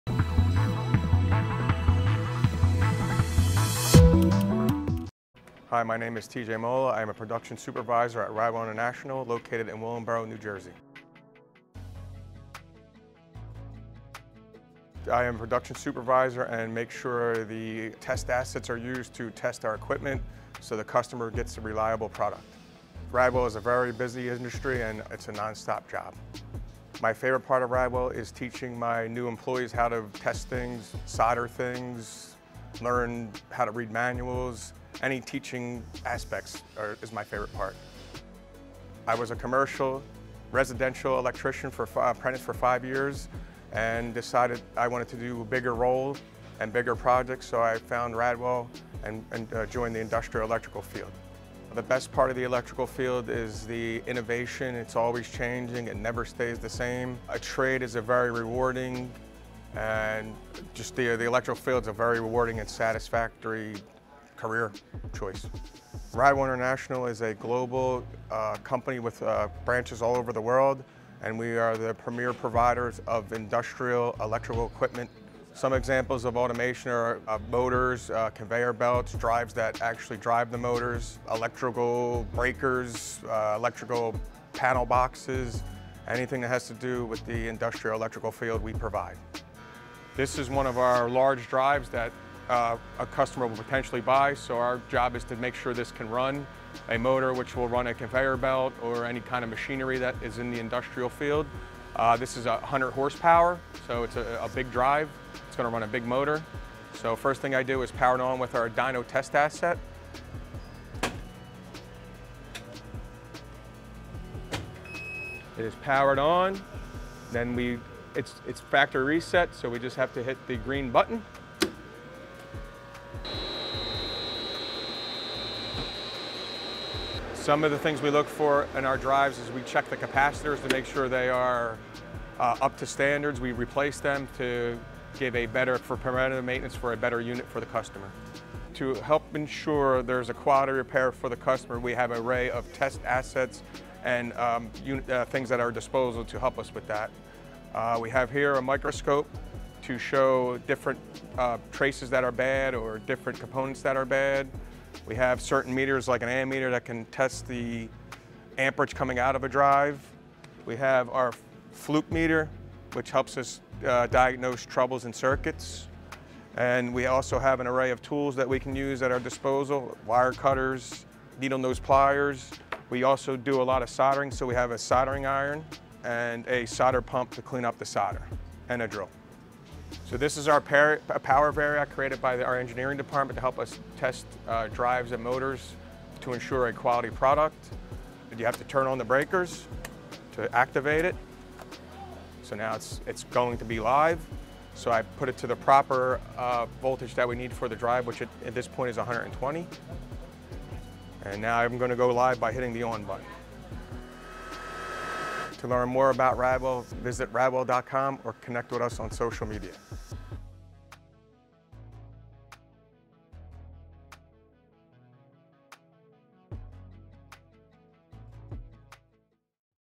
A Day in the Life: T.J. Mola (Pre-Sales Production Supervisor)
Learn about a day in the life of our Pre-Sales Production Supervisor, TJ Mola, in this latest installment to our Day in the Life series from Radwell TV.